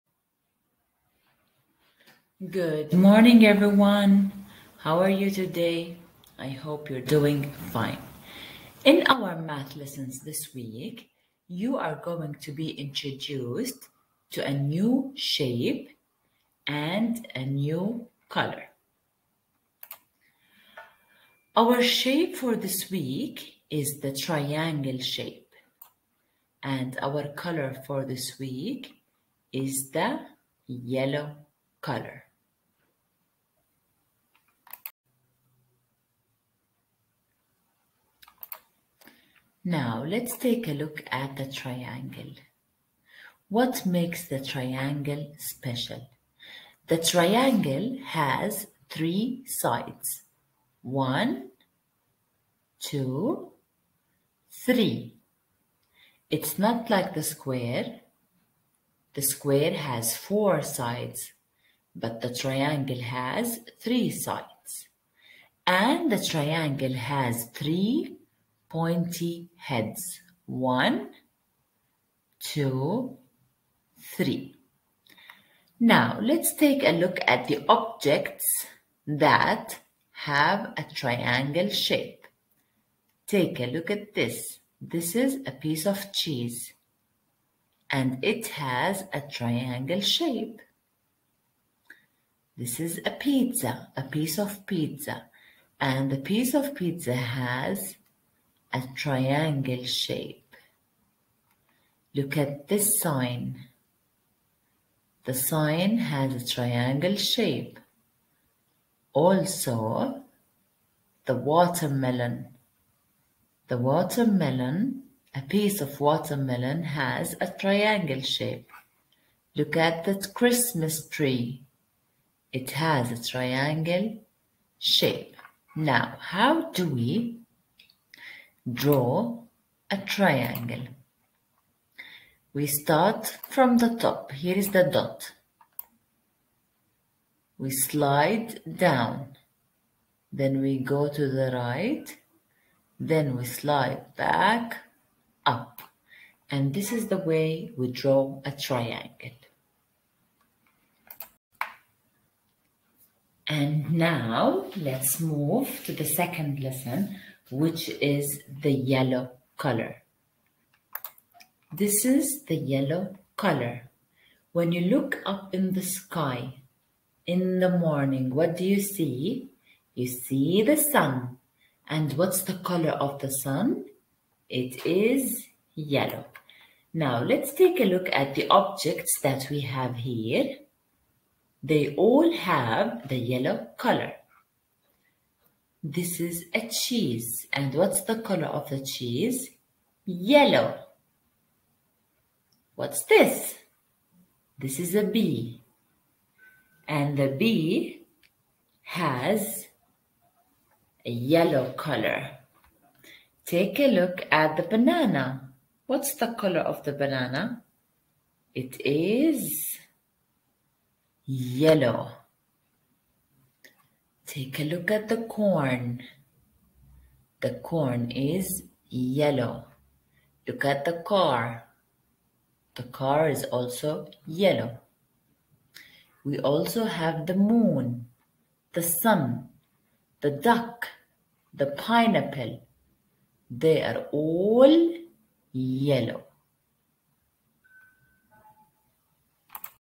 The triangle shape and the yellow color - Math - KG1 - Week 6 - By Ms. Tala Awamleh
Math:

The triangle shape and the yellow color

Students will be introduced to the triangle shape and the yellow color. Students will learn the formation of the triangle shape  and will recognize objects with a yellow color.